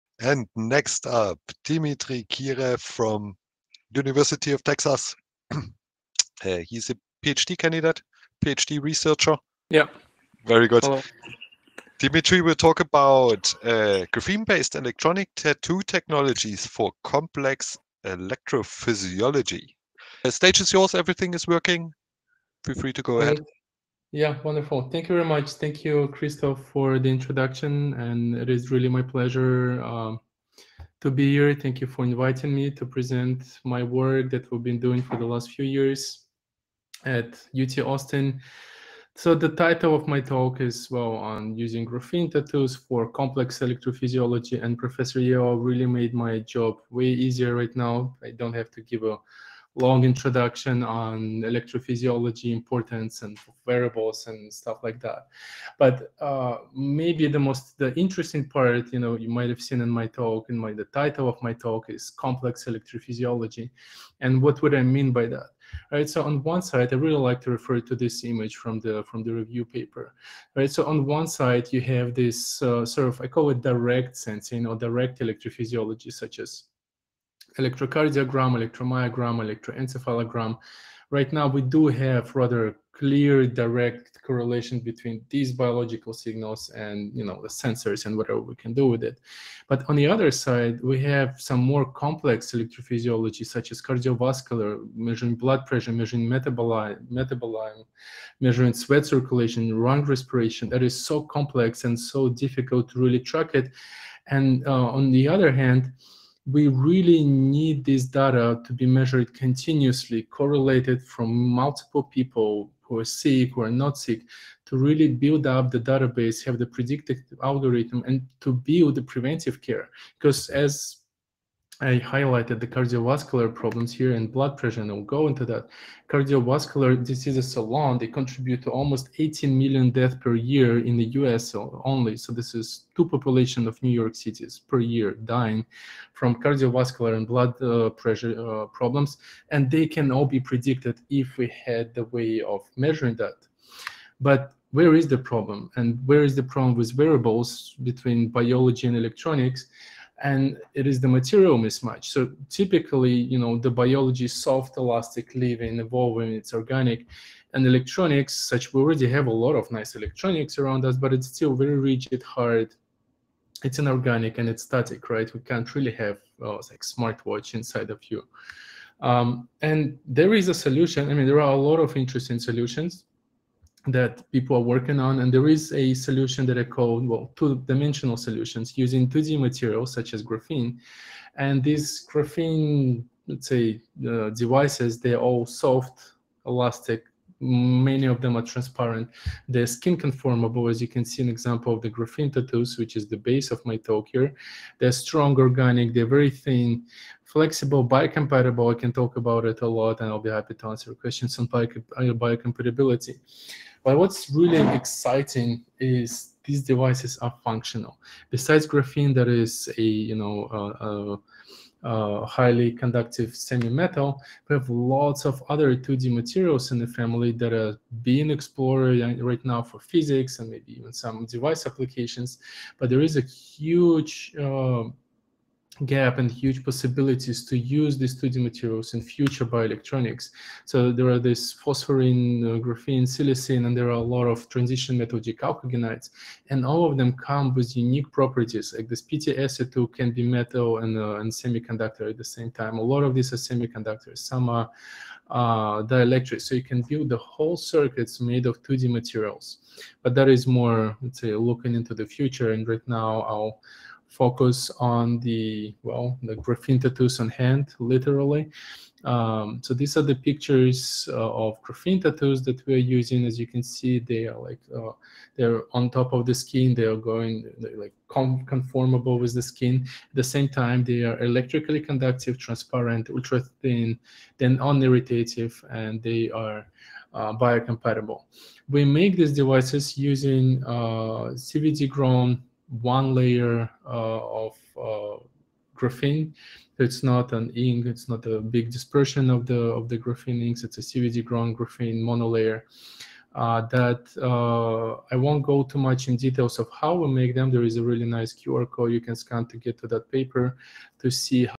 University of Texas | Graphene based electronic tattoo technologies for complex electrophysiology
To watch this presentation in full, please purchase at
TechBlick Annual Pass and login to

Dmitry Kireev, Research Associate

Monitoring complex health-related electrophysiological signals such as arterial blood pressure (BP) in ambulatory settings is essential for a proper understanding of health conditions, predominantly cardiovascular diseases. Moreover, continuous long-term monitoring of BP for patients with sleep apnea, stroke, or hypertension is essential to understand their health risk factors and build preventative care routines. While conventional ambulatory BP monitoring devices exist, they are uncomfortable, bulky, and intrusive. The common drawbacks of all these systems are their bulkiness and incompatibility with skin’s elastic properties, causing sensor’s displacement during usage, consequently requiring frequent system re-calibration.
In our work, we introduce a unique wearable BP monitoring technology that leverages atomically-thin and electrically conductive graphene electronic tattoos (GETs) as main building blocks. The GETs are placed over the radial and ulnar arteries on the wrist and subsequently used as current injection and voltage sensing electrodes, measuring arterial bioimpedance. In contrast to any other wearable system, the atomically thin, lightweight, and skin-conformable GETs do not apply any external tension onto the skin during the operation. Hence, they are able to perform long-term and nocturnal measurements without discomforting the subjects. Using bioimpedance modality allows us to disregard the tattoo-skin interface, which is typically 2-4 orders of magnitude larger compared to tissue impedance, and record only from the areas of interest. Employing a machine learning regression model on the recorded bioimpedance value, we yield effective beat-to-beat detection of diastolic and systolic BP values with grade-A accuracy. Besides BP, we show that the same Bio-Z signal can be post-processed to estimate person’s RR in an entirely wearable and non-invasive manner.